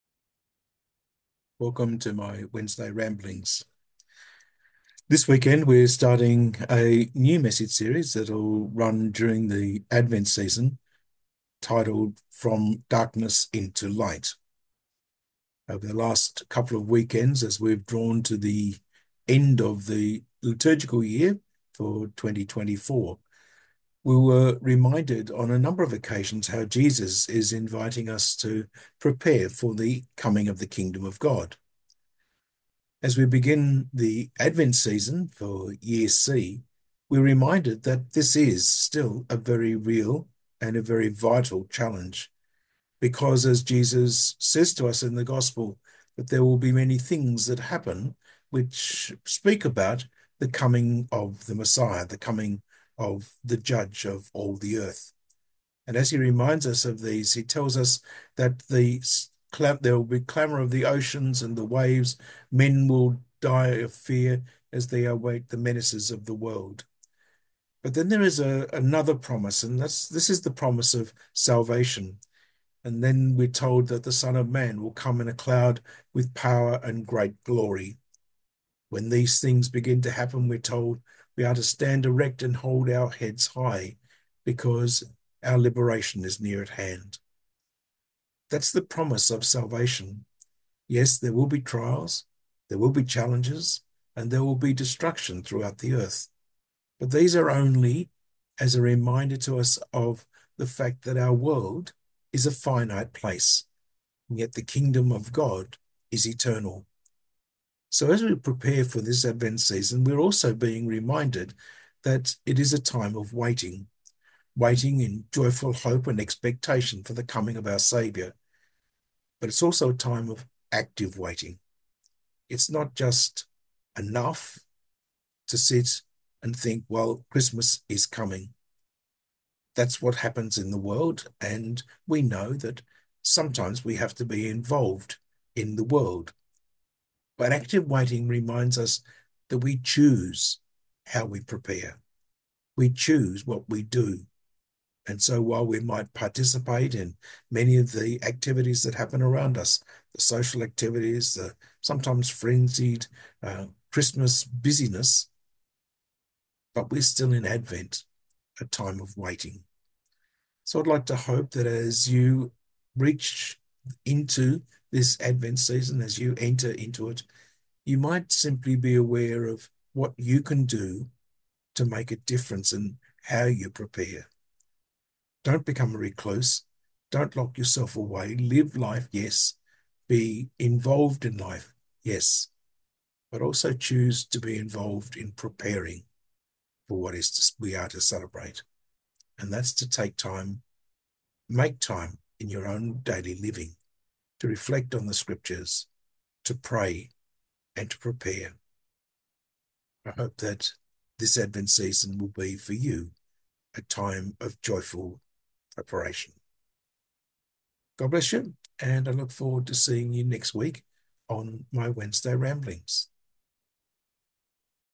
Fr Mike's Wednesday Ramblings (27th November 2024)
Fr Mike shares some thoughts on the 1st week of his Advent Message series - From Darkness into Light